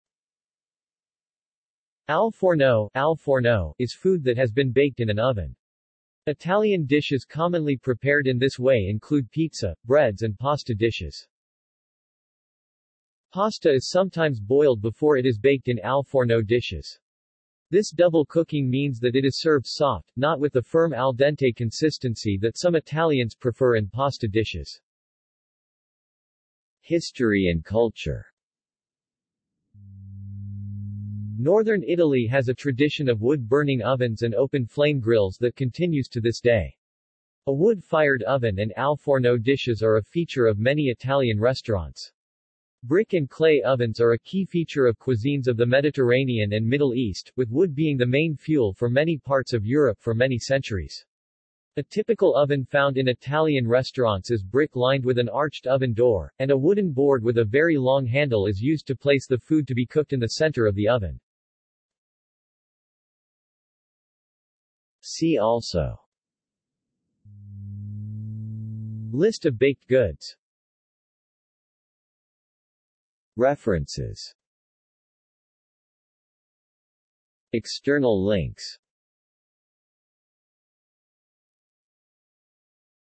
Al forno | Wikipedia audio article
This is an audio version of the Wikipedia Article:
Al forno

SUMMARY
Al forno [al ˈforno] is food that has been baked in an oven. Italian dishes commonly prepared in this way include pizza, breads and pasta dishes.
Pasta is sometimes boiled before it is baked in al forno dishes. This double cooking means that it is served soft, not with the firm al dente consistency that some Italians prefer in pasta dishes.